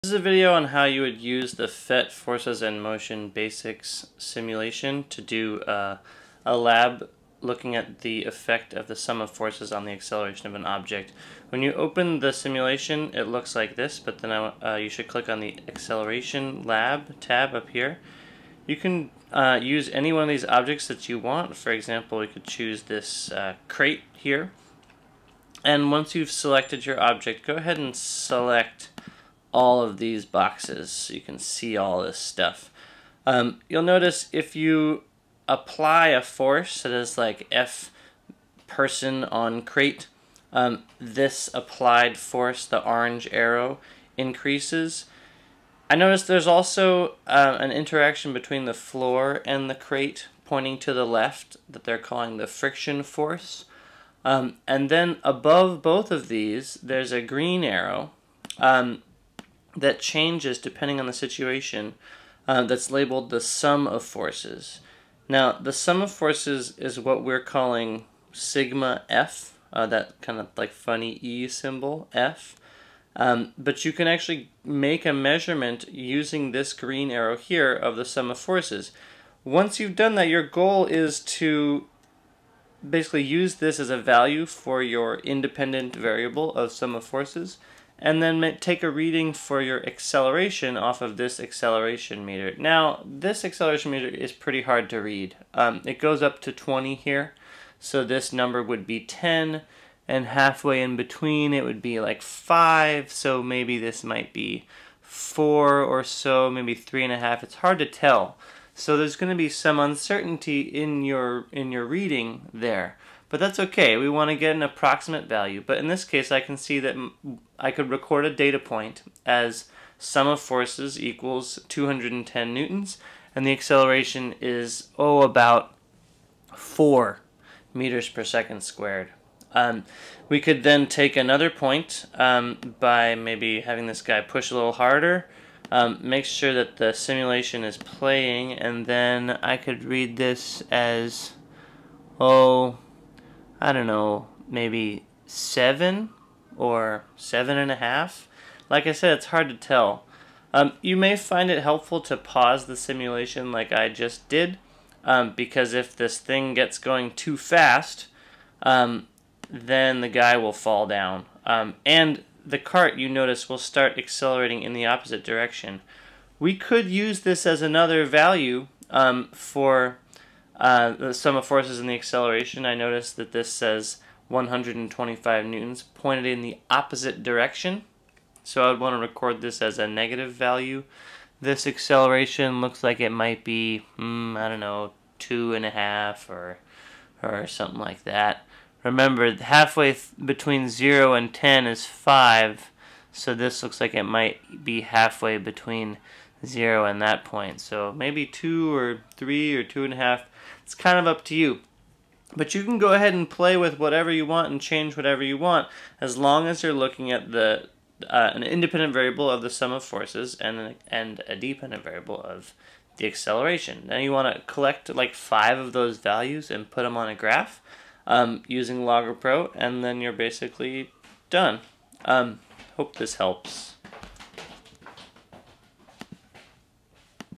PhET Force And Motion Basics Acceleration
A quick tutorial on how to use the PhET Force and Motion Basics simulations to do a quick experiment on the relationship between ∑F and acceleration.